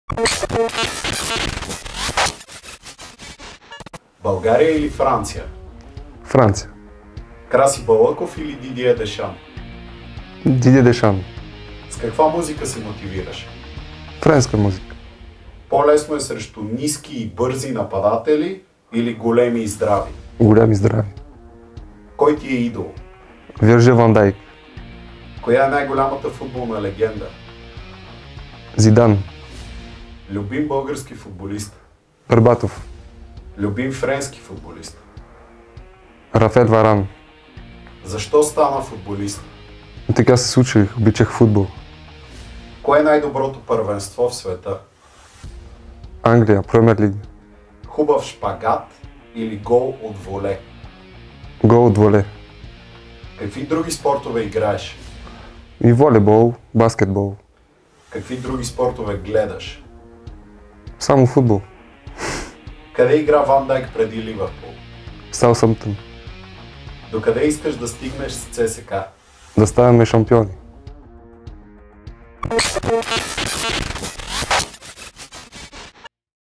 "Бързи 15" със Симеон Петров
Симеон Петров се изправя срещу „Бързи 15“ – рубриката, в която играчите на ЦСКА открехват вратата към личния си живот, интересите си и любимците си във футболната игра.

Централният защитник, израснал във Франция, трябваше да реагира бързо, както и често му се налага на терена в сблъсъците с противниковите нападатели.

За Франция и България, за Дешан и Балъков, за шпагатите и волетата – Симеон Петров.